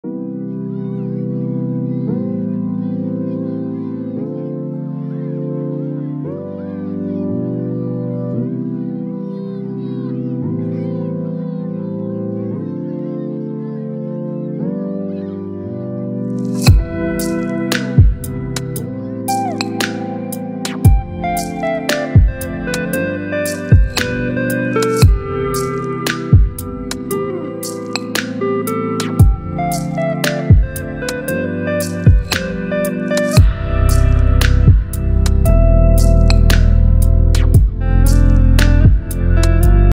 My cat is dreaming
Sometimes Bread can be a deep sleeper.